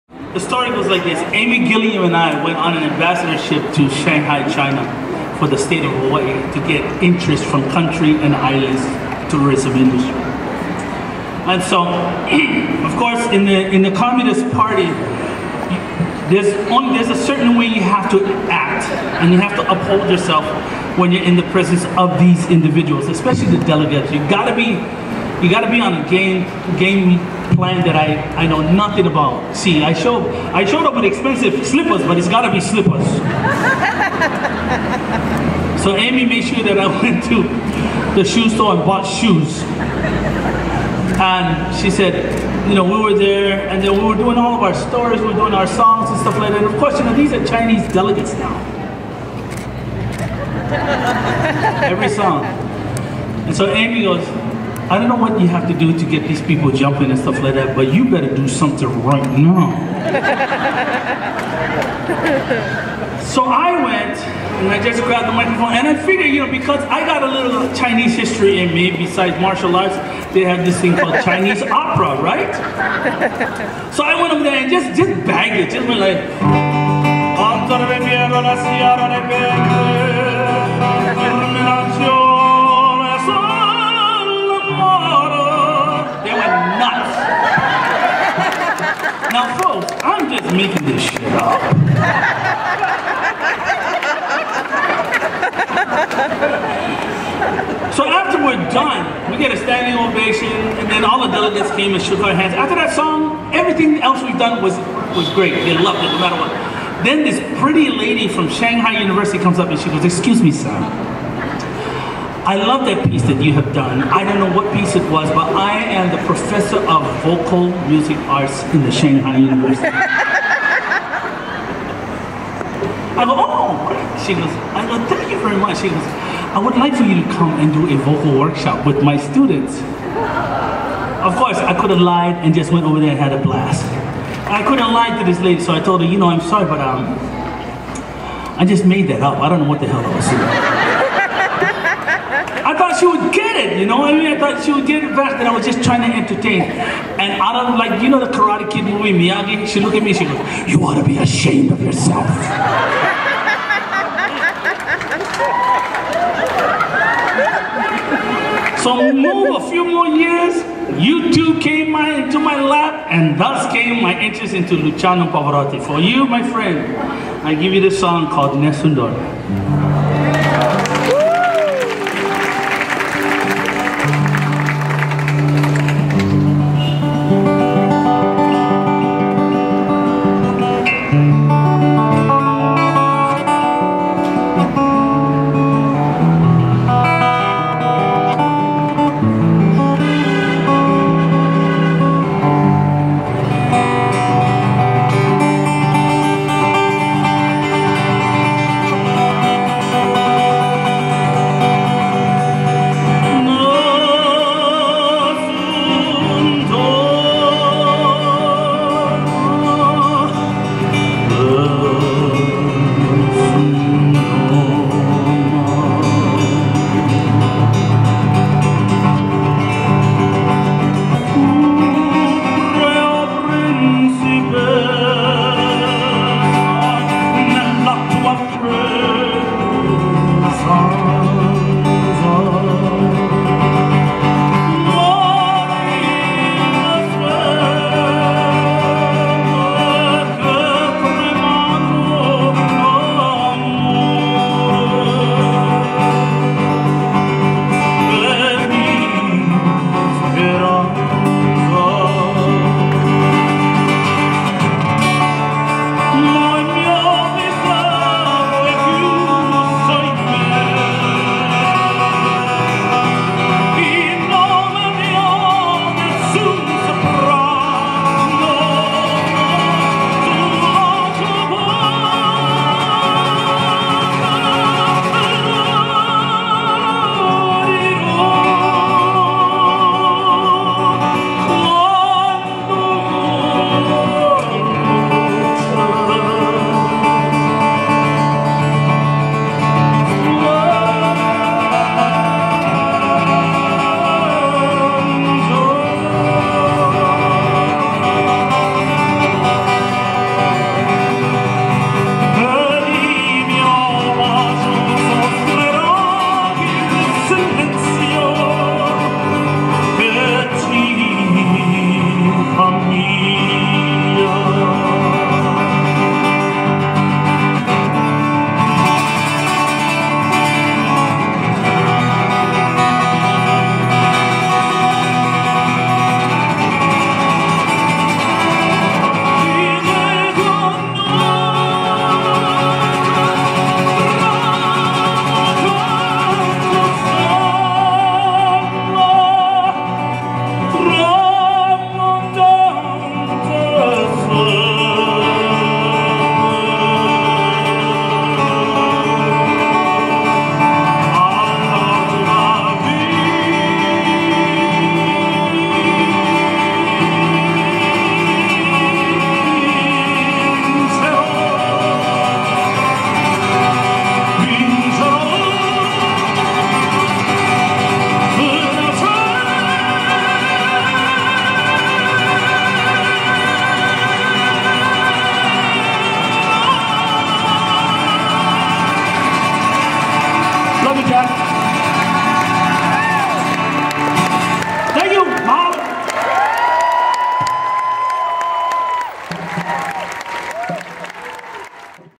Willie K Sings Luciano Pavarotti - Love Makes A Family
Willie K follows up a humorous story with a virtuoso opera performance at the Love Makes A Family event at the Hawaii State Capital.

Mahalo in advance!

uncle willie
hawaiian music
Willie K